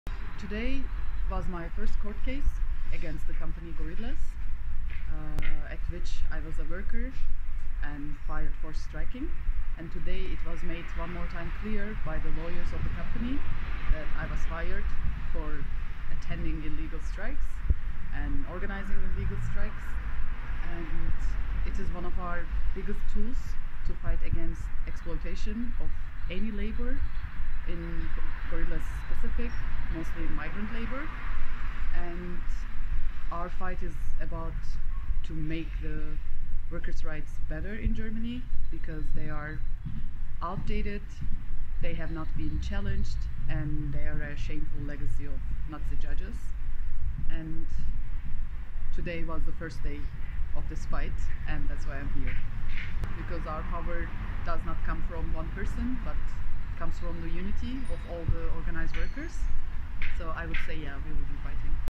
Duygu (Gorillas): "Unsere Kraft kommt von der Einheit der organisierten Arbeiter:innen"
Duygu, gefeuerte Arbeiterin und Streikanführerin von Gorillas, im Interview am ersten Tag ihres Gerichtsverfahrens gegen ihre Entlassung am 8. November 2021.